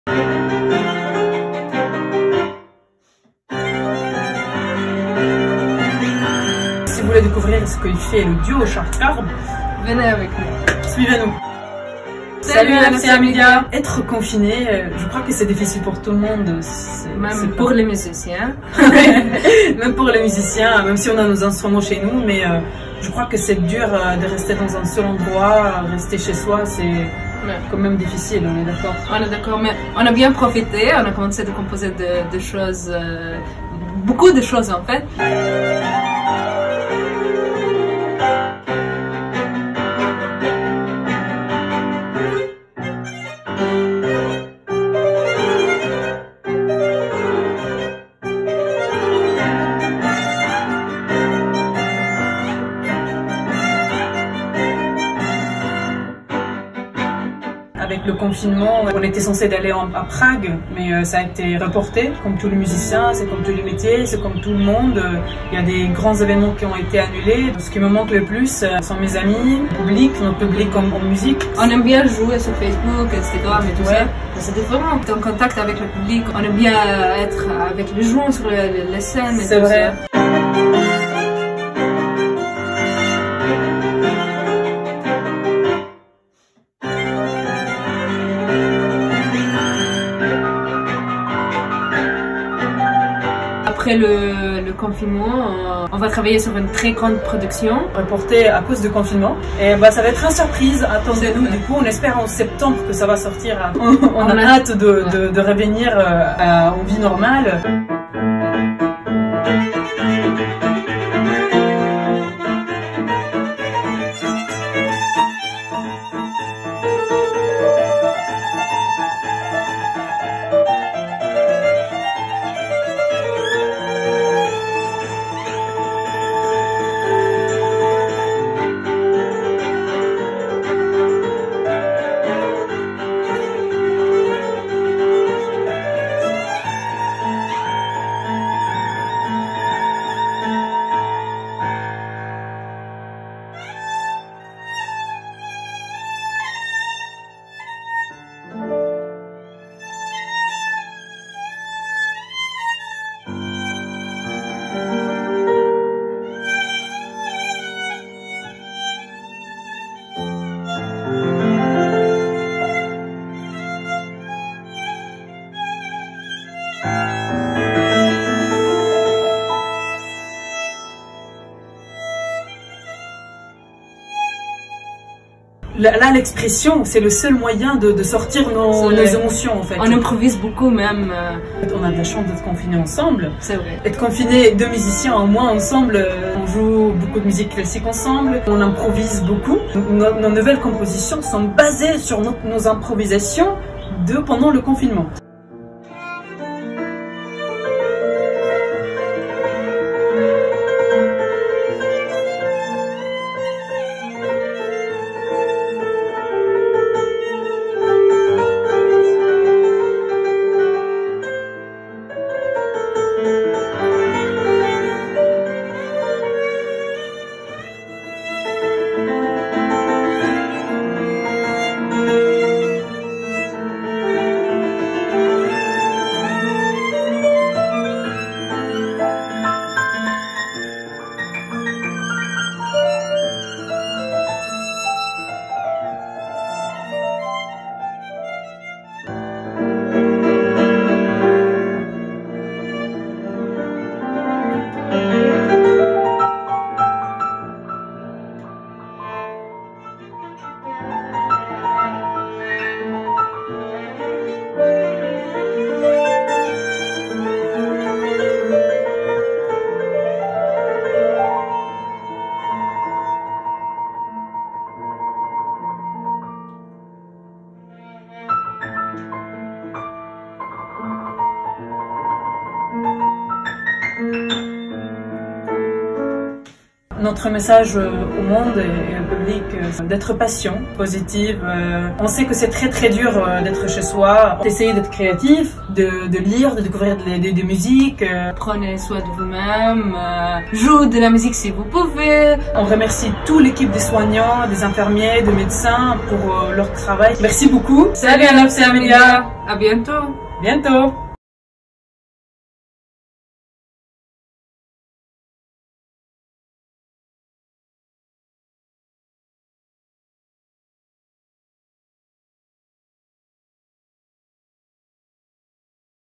🔎 Charq Gharb : Quand la musique classique rencontre l’orient.
Decouverte Talent sur NFCA MEDIA
Quand la musique classique rencontre l’orient

Mira Abualzulof au piano et Lamar Elias au violon forment le duo baptisé "Charq Gharb" qui signifie Orient-Occident.
Ces Deux jeunes Palestiennes fraîchement sorties du Conservatoire de Toulouse nous ont accordé cette interview et une prestation inédite.
Envoûtant et fort en émotion, c’est le coup de ❤️ du jour!
Dites-nous ce que vous en pensez...